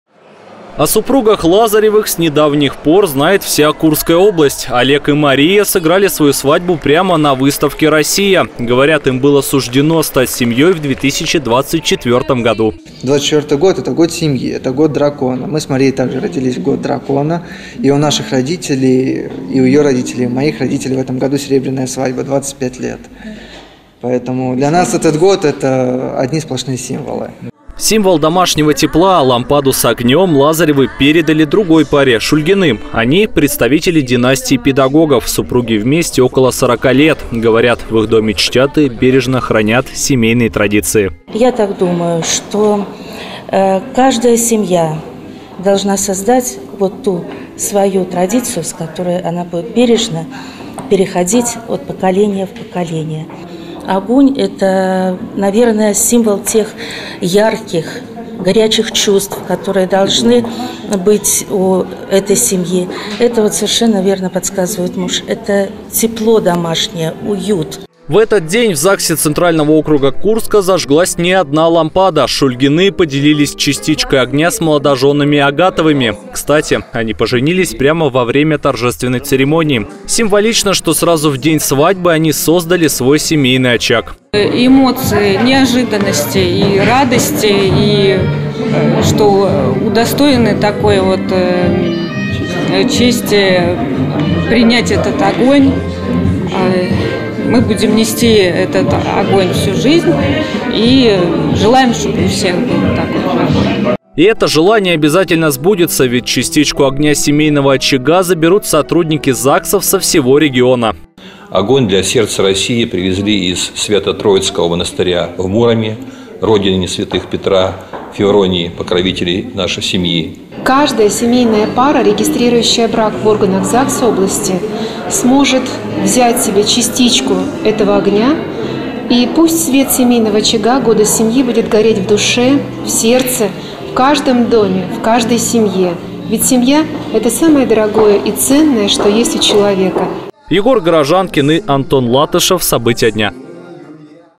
В Курск привезли огонь семейного очага «Сердце России», который зажгли в Москве на ВДНХ
В Курск привезли огонь семейного очага, тот самый, который зажгли в Москве на выставке-форуме «Россия». В областной центр лампаду доставили молодожёны Лазаревы, которые сыграли свадьбу там же, на ВДНХ. Они пЕредали частичку тепла другим семьям региона. Пламя будет храниться не только у них дома, но и во всех ЗАГСах Курской области.